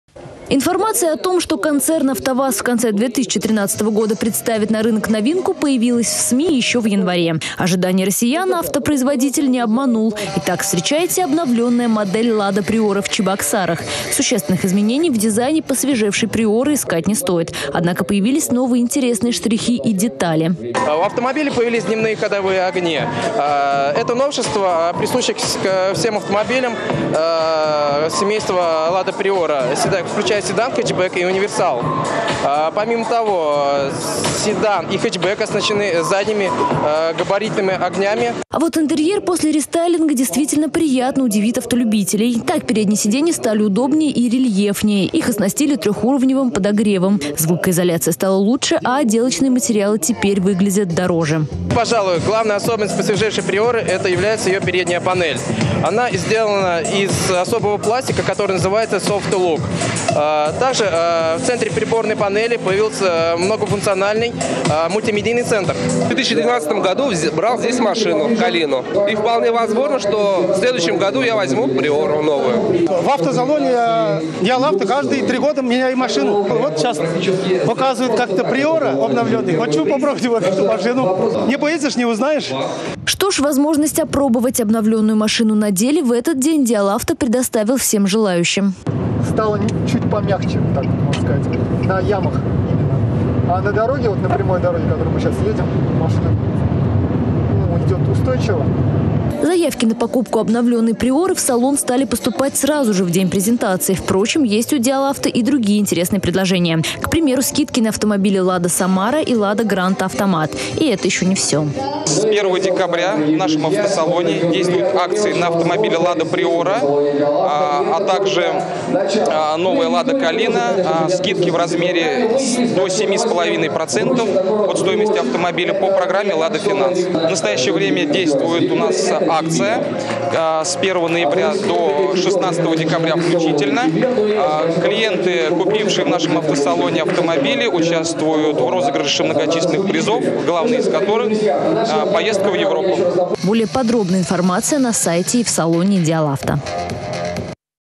Обновленная LADA Priora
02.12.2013
Судя по всему, те времена, когда с конвейеров советского автопрома десятилетиями могли сходить одни и те же модели без изменений, прошли. Теперь автомобили «освежают» раз в несколько лет. Полгода назад АВТОВАЗ представил новую LADA Kalinа, ну а теперь настало время «Prior'ы». В минувшие выходные презентацию для жителей республики устроил дилерский центр  LADA «Диал-Авто».